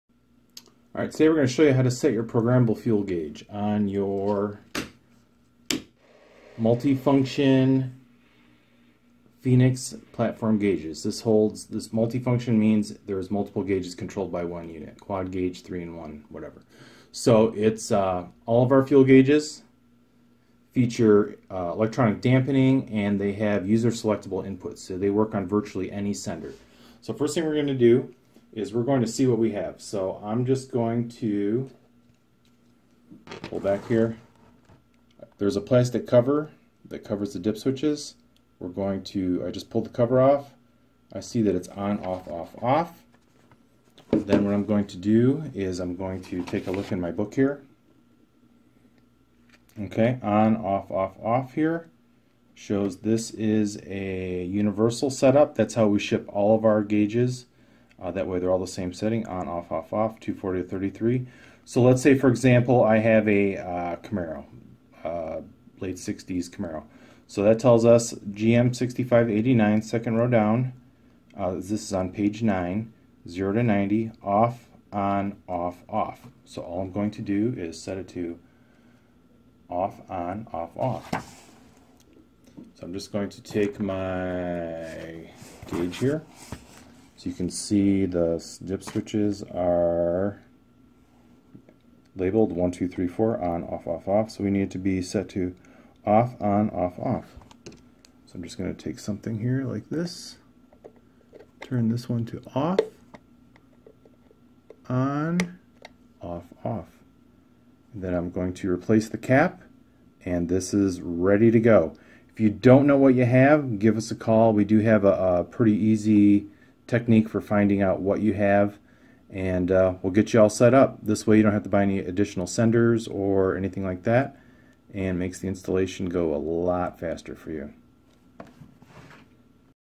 Phoenix Programmable fuel gauge
How to easily set up your Phoenix Platform programmable fuel gauge.